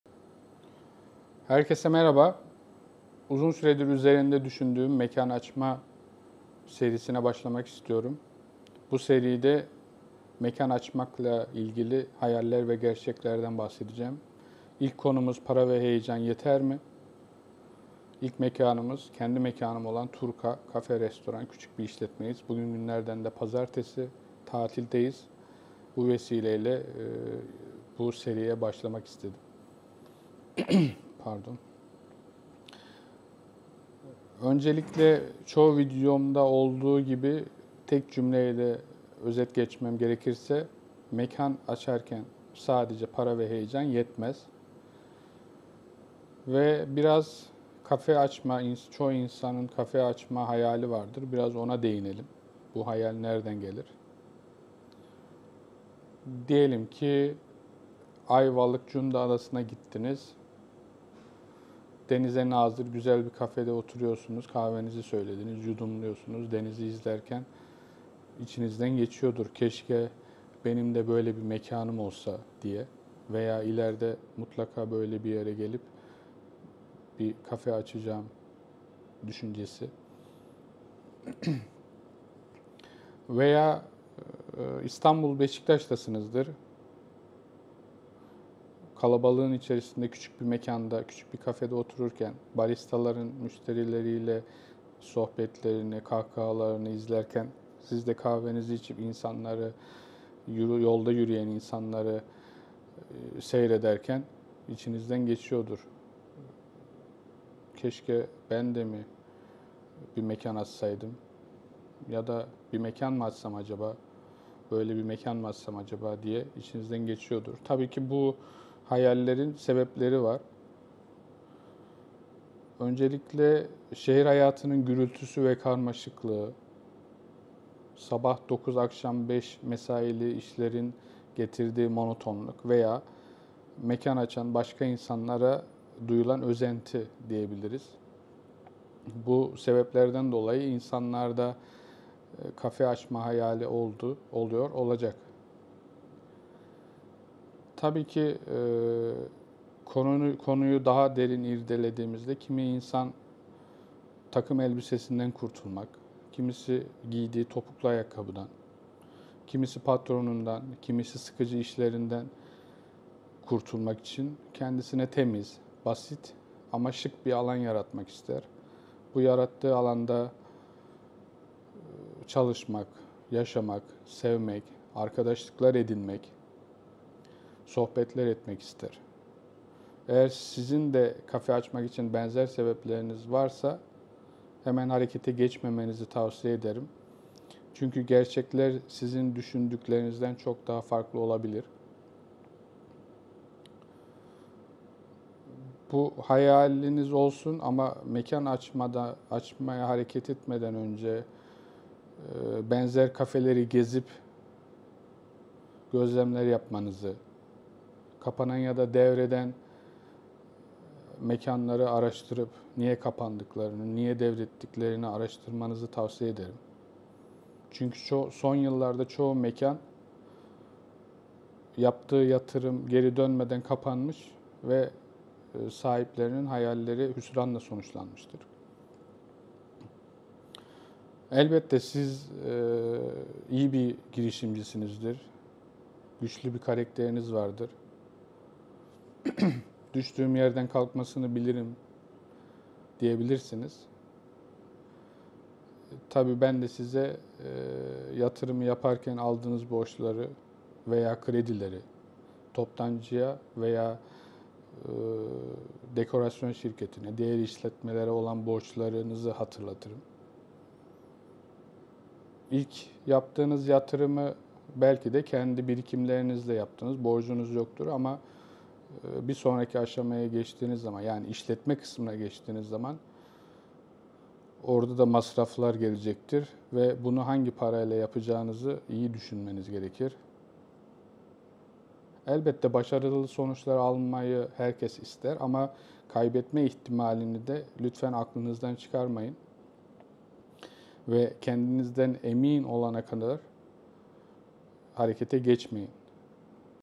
MEKAN AÇMAK - Para ve Heyecan Yeter mi? Kafe Açma Hayali
Merhabalar,
Mekan açmak ile ilgili bir seriye başlıyorum ve bu seride elimden geldiğince Hayallerden ve Gerçeklerden bahsedeceğim.
Daha çok mekan açma isteği, mekan açılımı ve işletmesi konularına değineceğim. Girişimci arkadaşlar için faydalı olacağına inaniyorum.
Saglıklı ve keyifli bir yaşam dilerim.